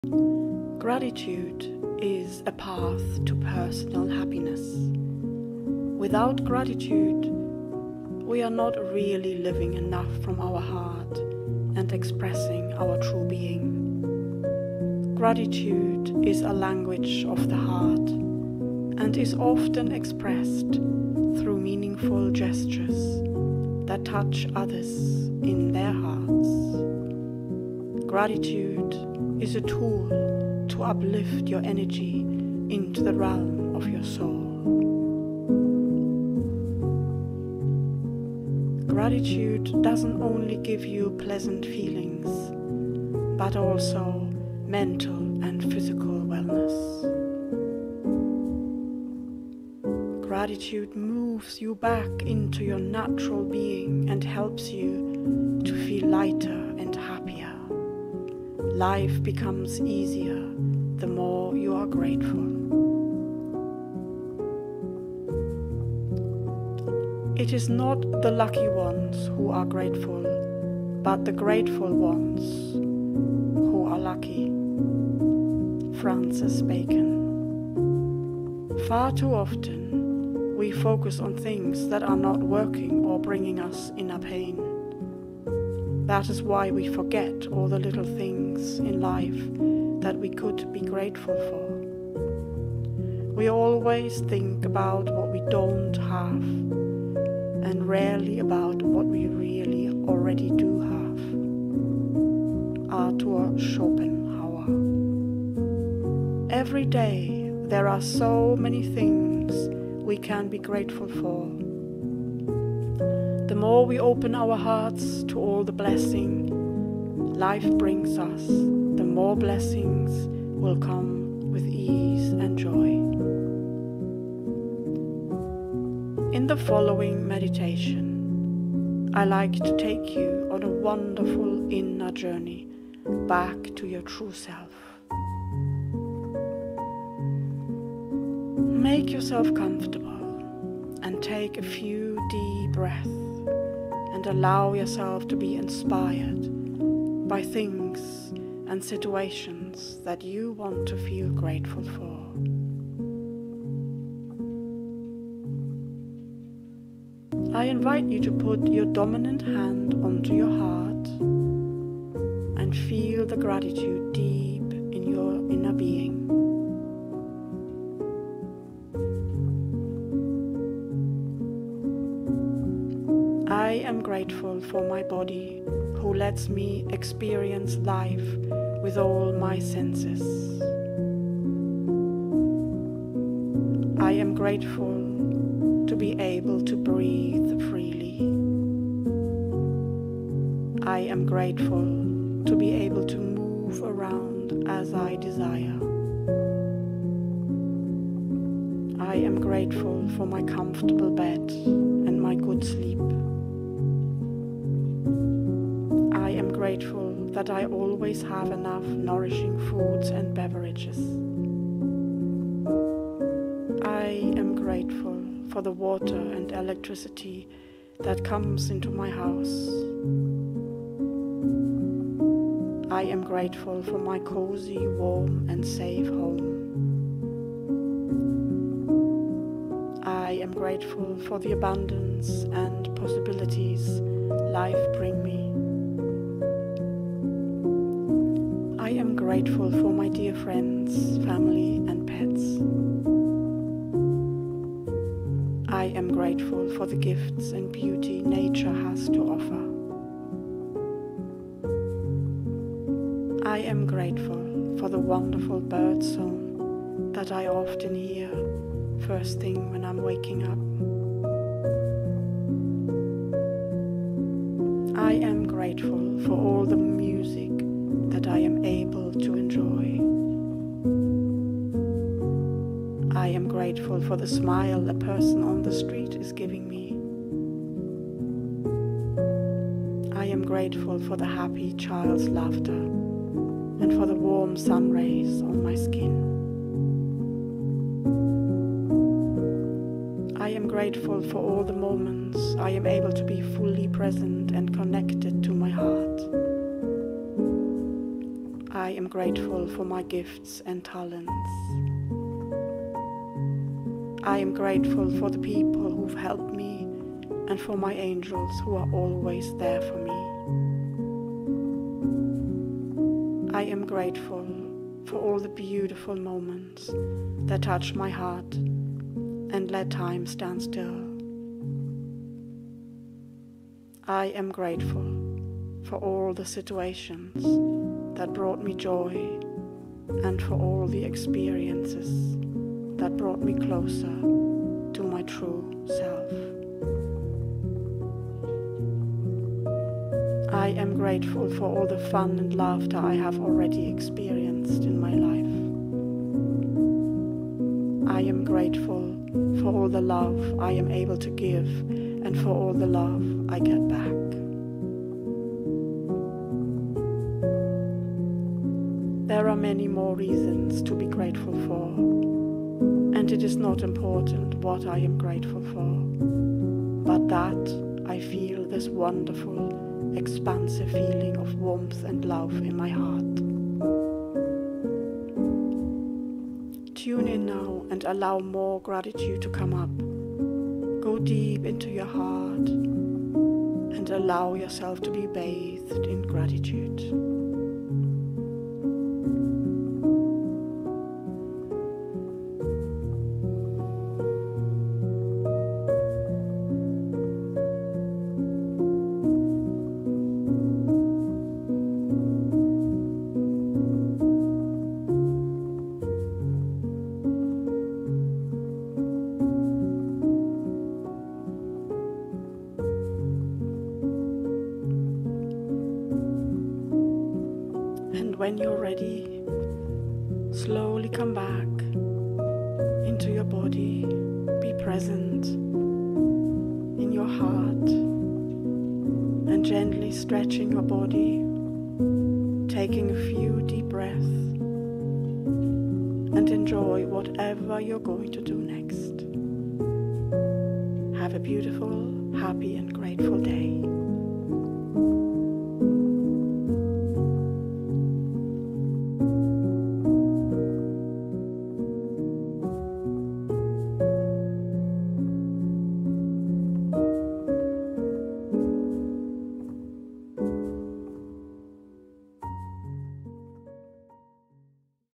OPEN your HEART to Gratitude. 8 Minute Meditation
Bath yourself in the energies of gratitude EVERY DAY to elevate your consciousness and create a more peaceful and happy life for yourself and everybody around you.

✅ INQUIRE TO WORK WITH ME